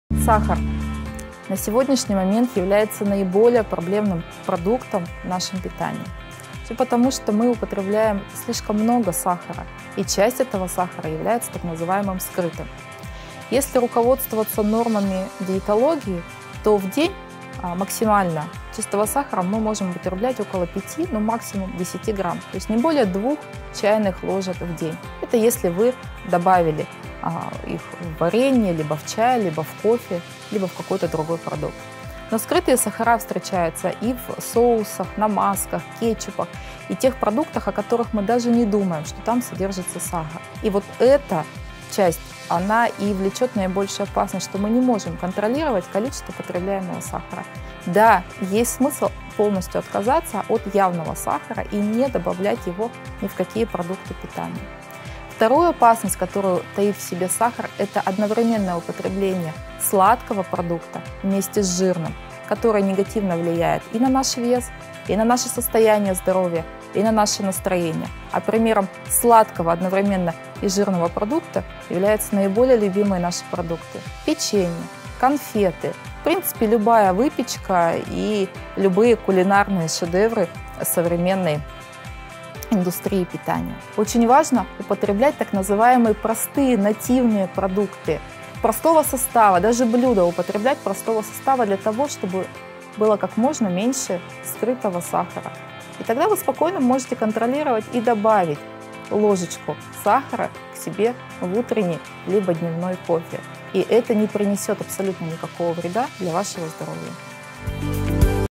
Врач-диетолог Любовь Щербаченко
Сколько сахара в день можно есть без вреда для здоровья?